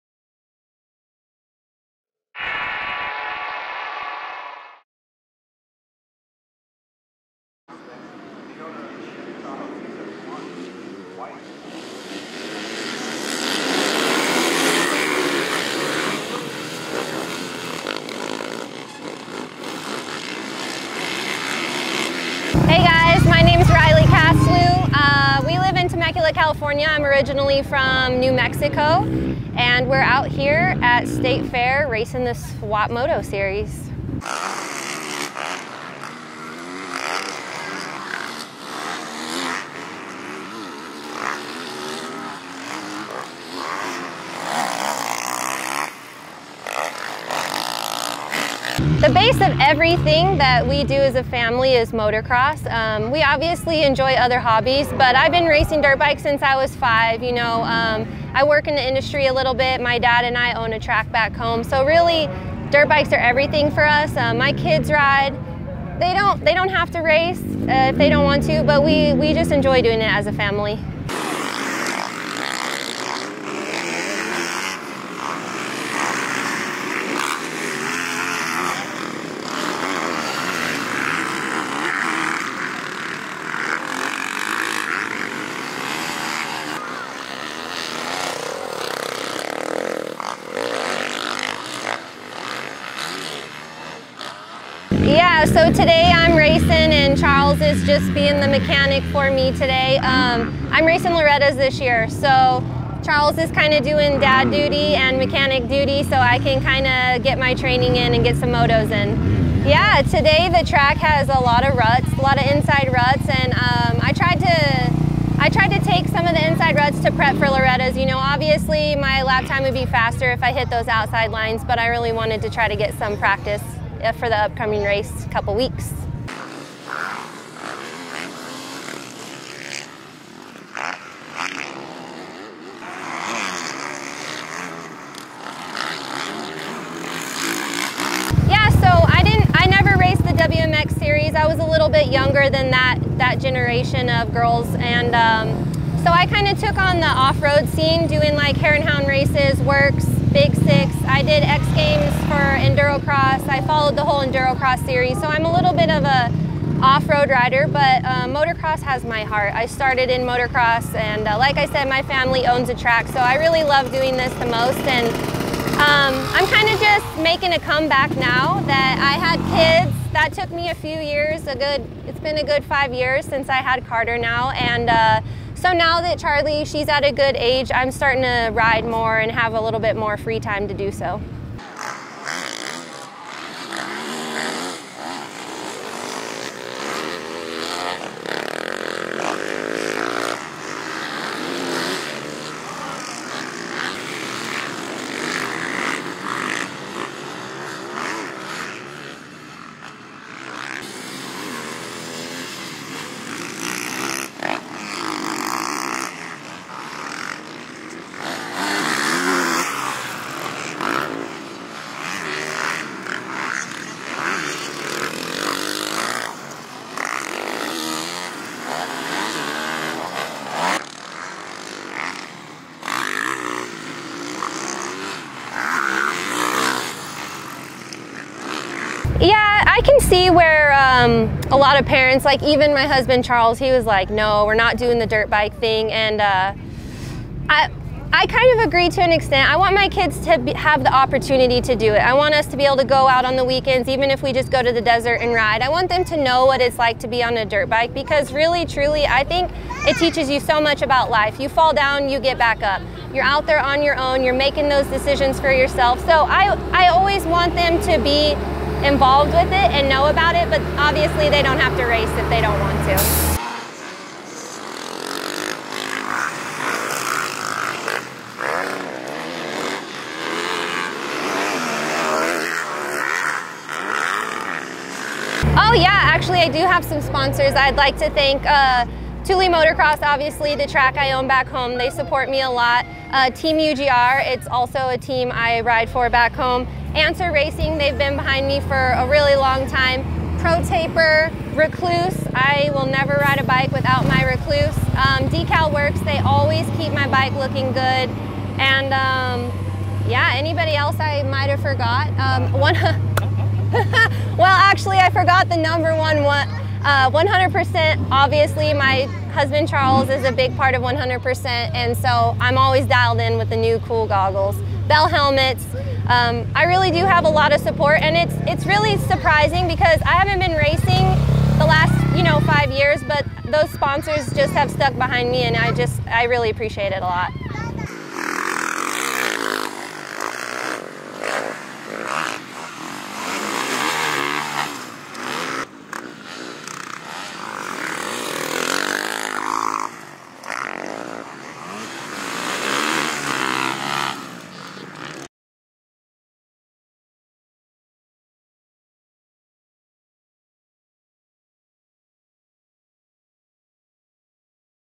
Rily Castloo Took Five Years Off to Start a Family, But She's Back on the Motocross Track!
The AMSOIL Swapmoto Race Series attracts racers from all walks of life, including moms who also love to moto! Rily Castloo is originally from New Mexico, but she's made a home in SoCal with her husband Charles, a former Supercross and National MX Pro, and their two young children. With her sights set on this weekend's Loretta Lynn's Amateur Nationals, Rily showed up to State Fair for the Terra Firma Series to tune up in some inside-line ruts!